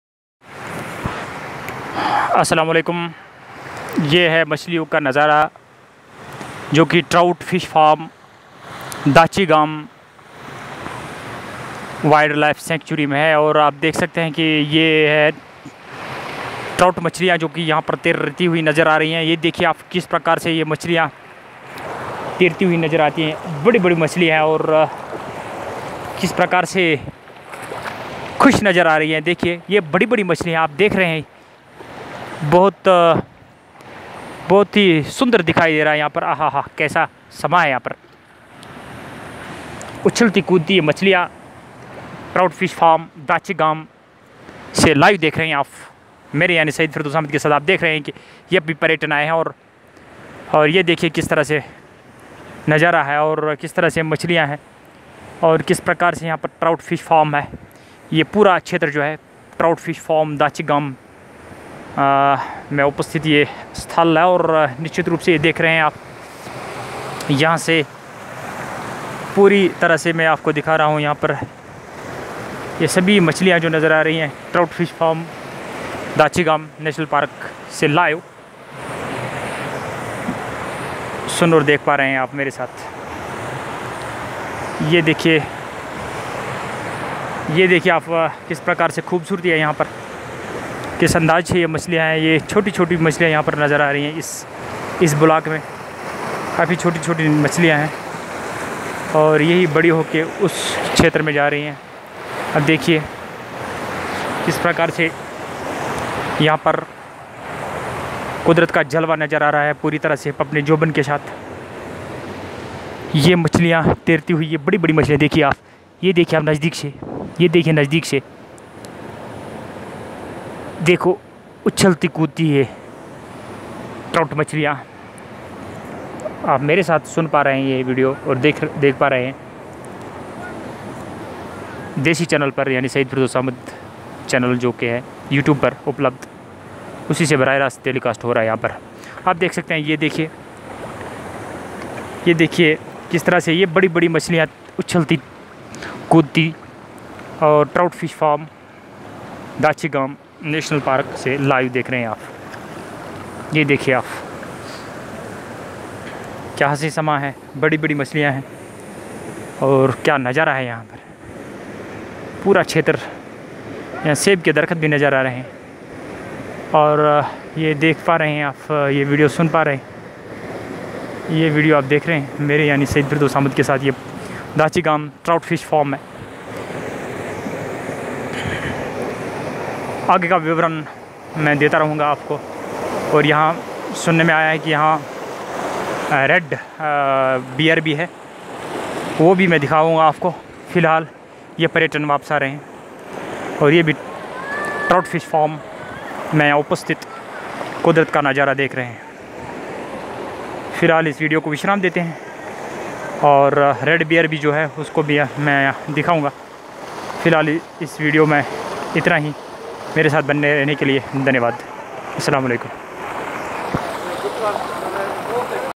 Awesome tours of SYED FIRDOUS AHMAD towards Dachigam National Park with live commentary........
Dachigam a winsome resort !